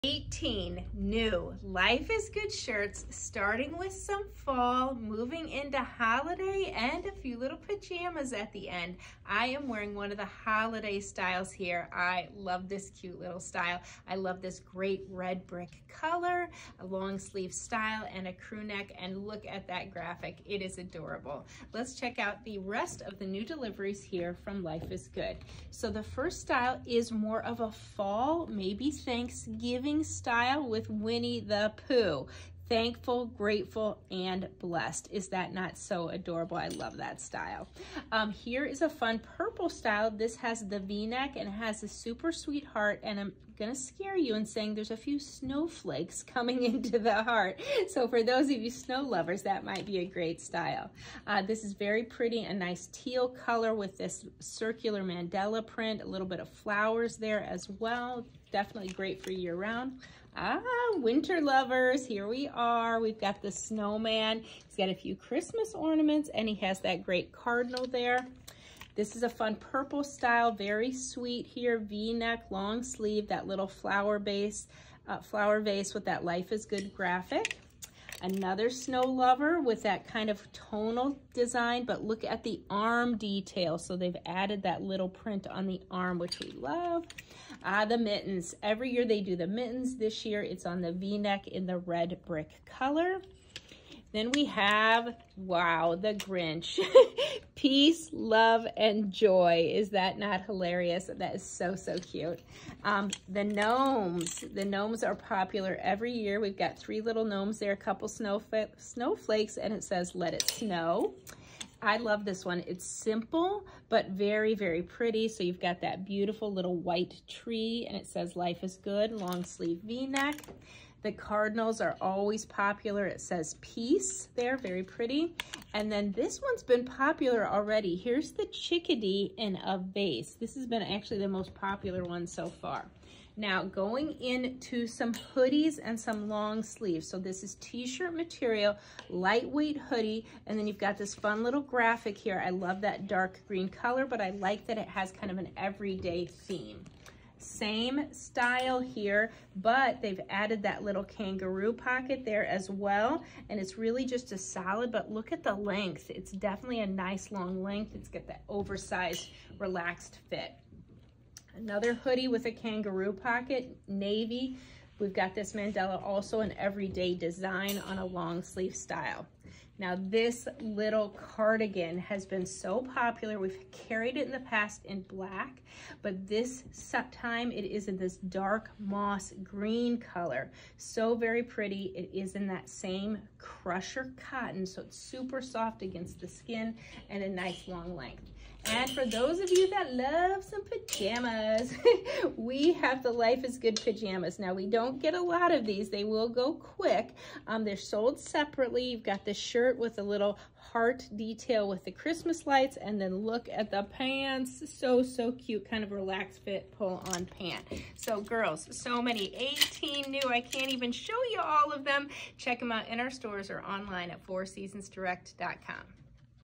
Life Is Good, Holiday and Winter Styles from FourSeasonsDirect.com
Four Seasons has so-o-o-o many new 🍁 fall, 🎄 holiday and ☃️ winter styles from Life is Good - that you won't be able to 🤔 Pick Just One!

There are long sleeved tees that feature some of our favorite characters like 🧸 Winnie the Pooh and 🐷 Piglet as well as the Grinch and his faithful 🐶 dog Max. But let's not forget the 🐦 chickadees, cardinals, ❄️❄️❄️ snowflakes, 🎄 Christmas trees and ☃️snowmen!

You can stay 🌡️ warm in the sweatshirts too! Some have hoods while others have 🦘 kangaroo pockets - there's even an open front cardigan in a new color - Dark Moss Green!

But the 📽️📽️ showstoppers are the 💤 jammie 💤 top with a Christmas Light 💗 Heart and the comfy Red and Green bottoms. Perfect for when you're waiting for 🎅 Santa!

You won't be able to 🤔 Pick Just One in this shipment from Life is Good!

SIZES - Small to XXXL
Code SPOOKY expires 10/29/2023